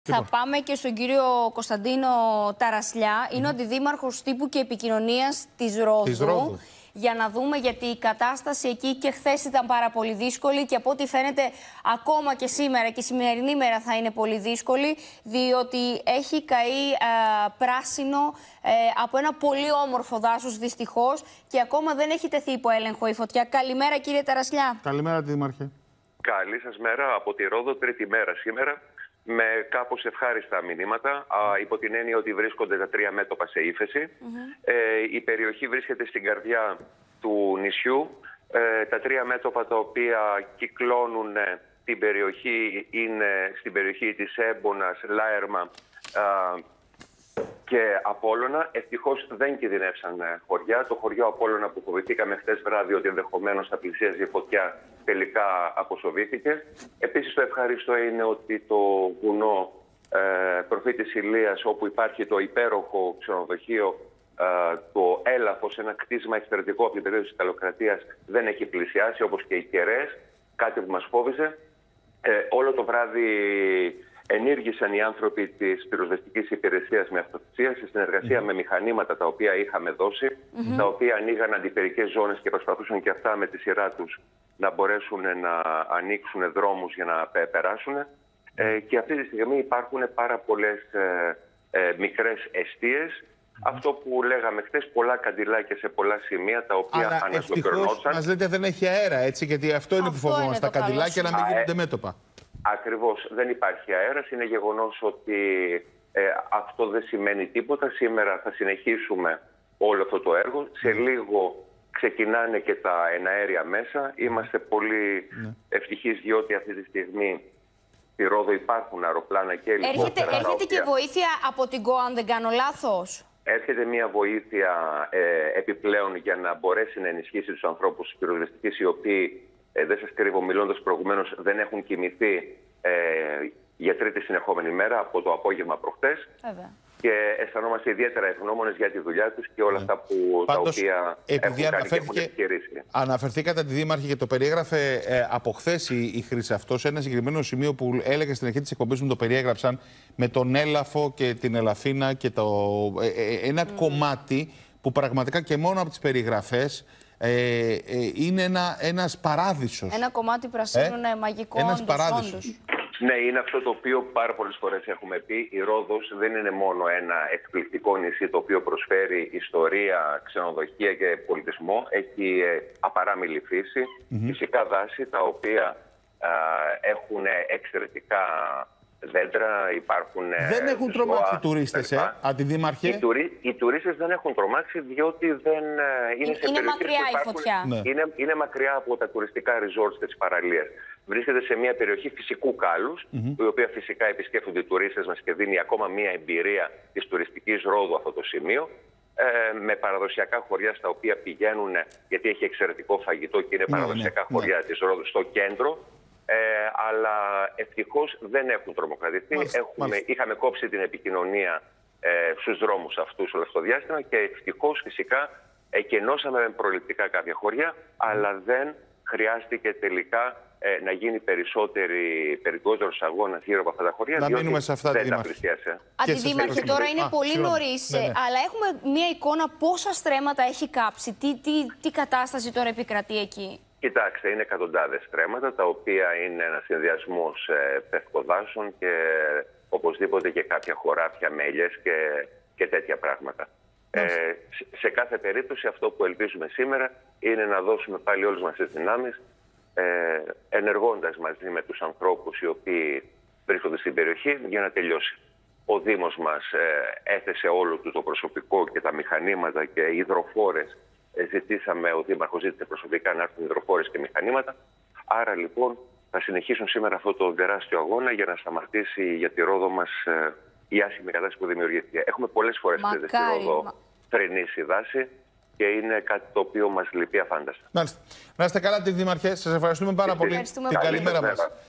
Ο Αντιδήμαρχος Τύπου και Επικοινωνίας Δήμου Ρόδου για την φωτιά στο νησί | OPEN TV
Γίνετε μέλος στο κανάλι του OPEN για να αποκτήσετε πρόσβαση σε προνόμια:
Σε ύφεση βρίσκονται και τα τρία μέτωπα της φωτιάς, σύμφωνα με τον Κωνσταντίνο Ταρασλιά Αντιδήμαρχο Τύπου και Επικοινωνίας Δήμου Ρόδου.